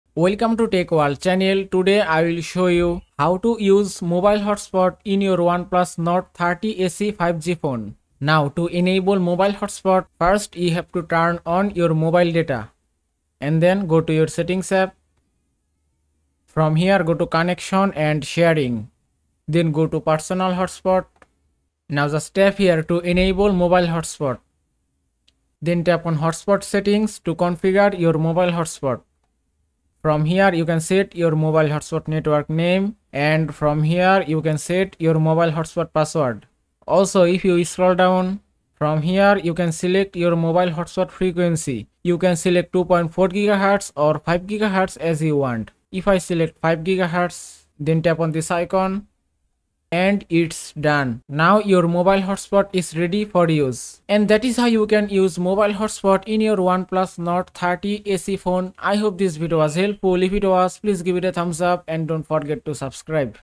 How to Setup Mobile Hotspot in OnePlus Nord N30 SE 5G
A quick guide to setup and use mobile hotspot in your OnePlus Nord N30 SE 5G phone.